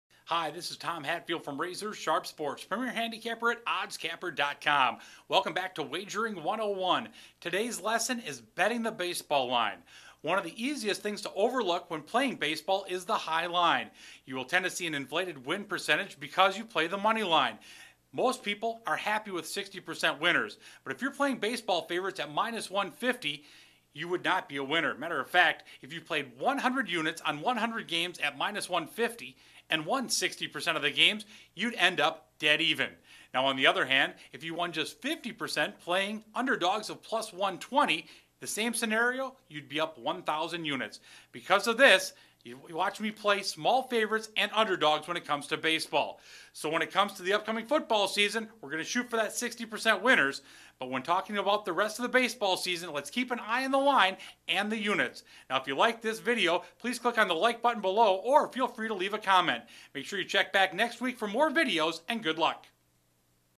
Wagering 101: Betting the line
Tom Hatfield, premiere handicapper at oddscapper.com, gives another quick wagering 101 lesson on betting the baseball line.